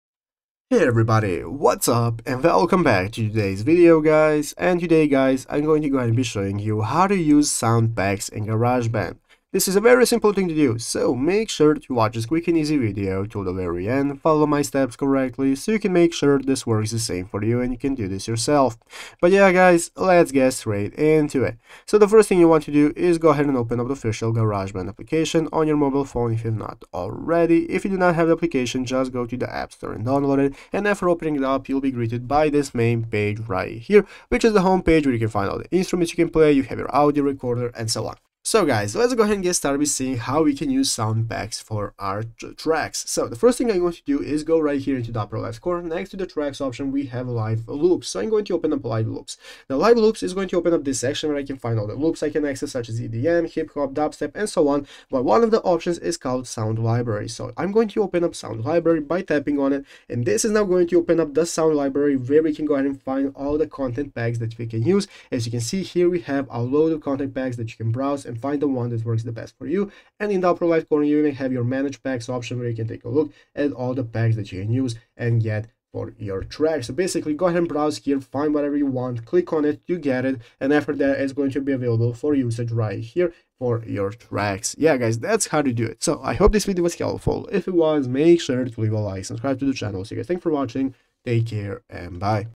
How To Use Sound Packs On GarageBand Tutorial
Today we talk about use sound packs on garageband,garageband sound library,garageband update,garageband ios,garageband sound packs